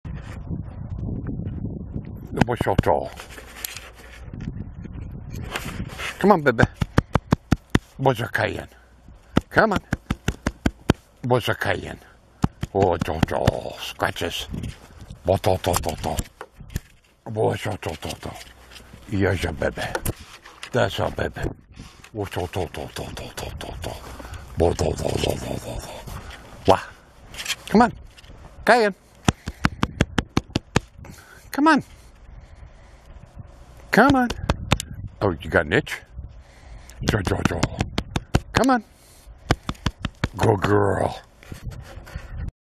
Llama Calling - Scratches
Who says you can't call a llama WITHOUT food in hand. Just thump your chest open handed and poof...Cayan comes for some scratches.